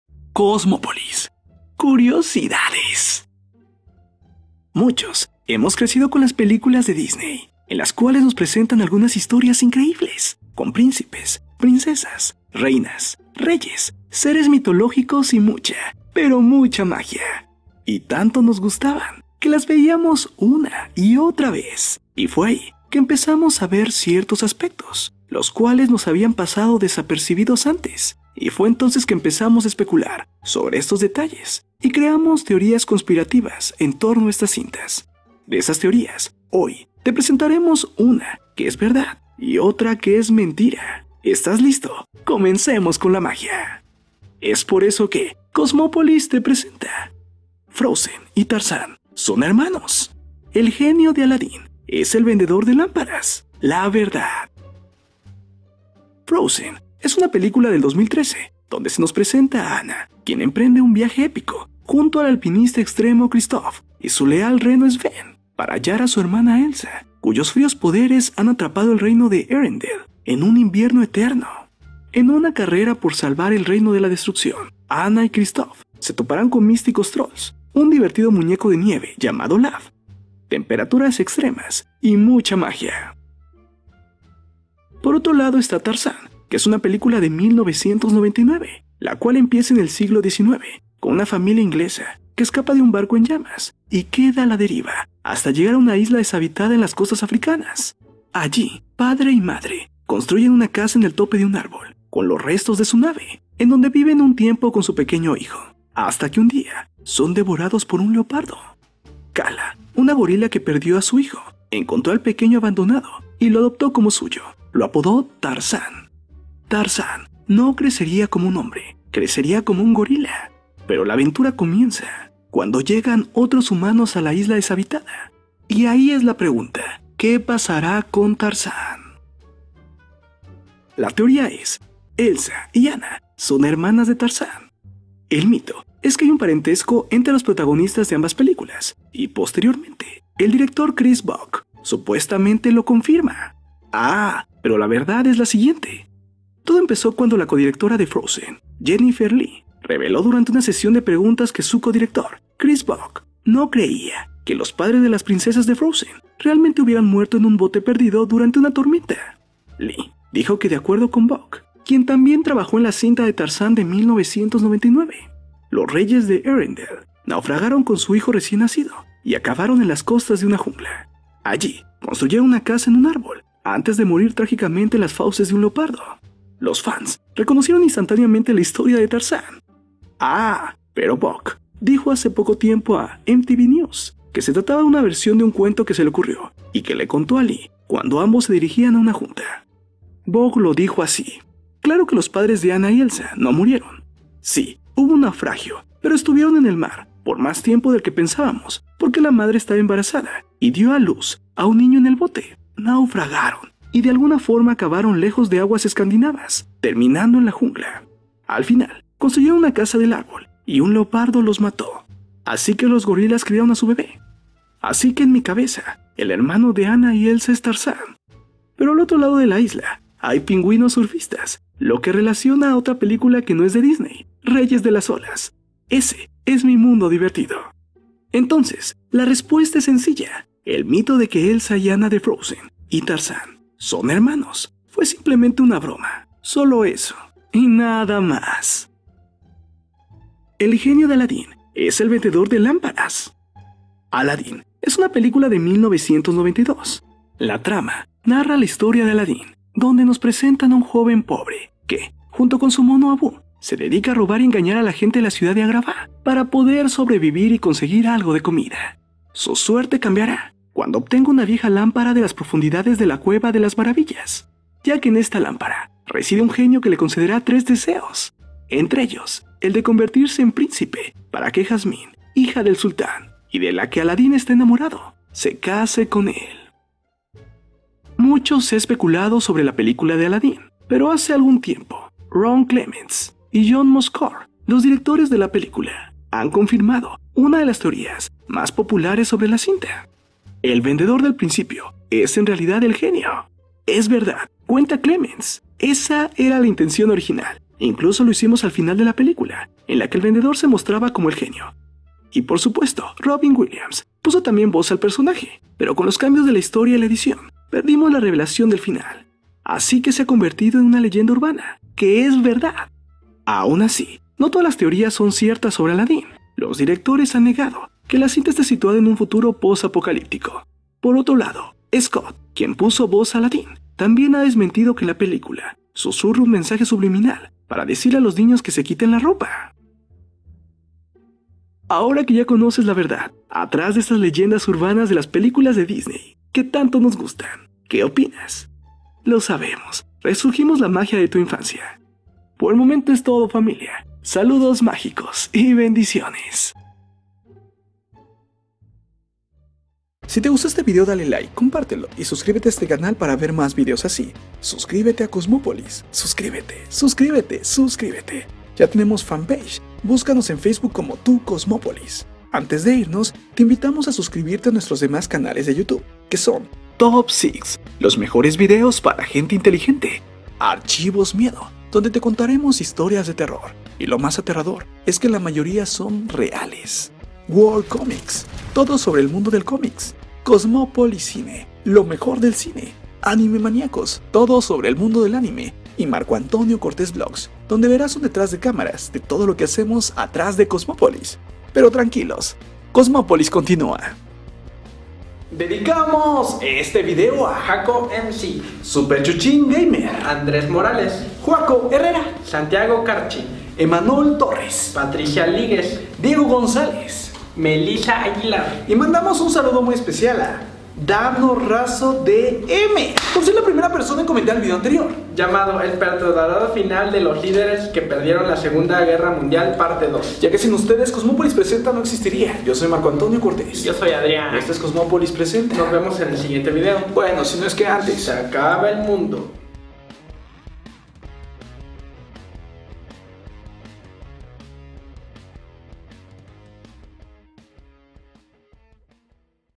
¿FROZEN Y TARZAN SON HERMANOS? ¿EL GENIO DE ALADDÍN ES EL VENDEDOR DE LAMPARAS? LA VERDAD
Ya somos más de 783,505 almas suscritas y unidas en COSMOPOLIS
782,336